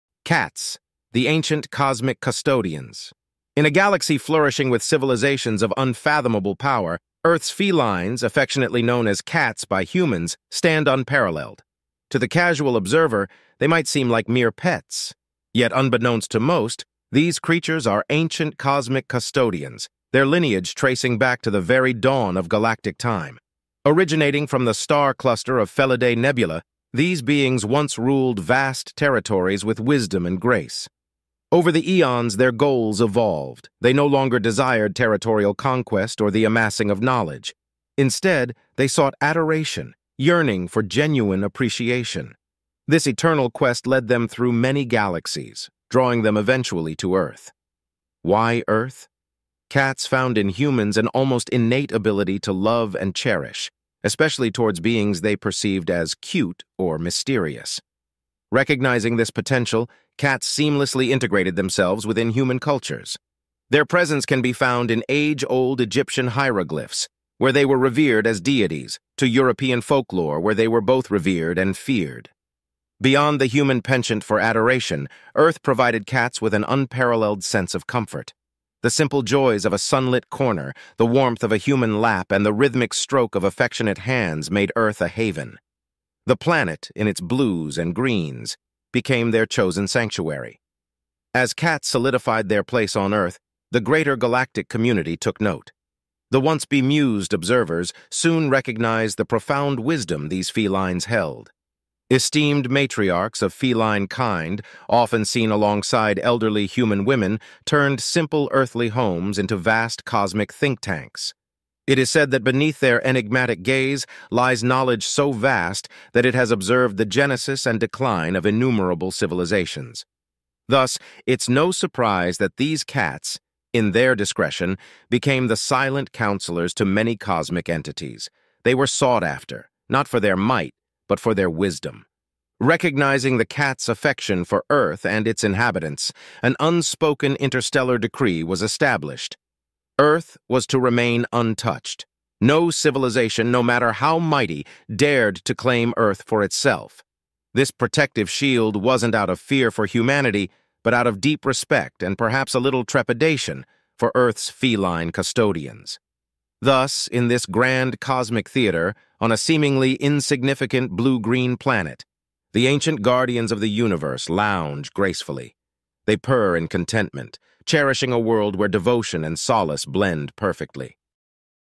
Cats: The Ancient Cosmic Custodians | A Short Sci-Fi Story
Do you have a cat? Now you know their secret!

Story by: u/scifistories1977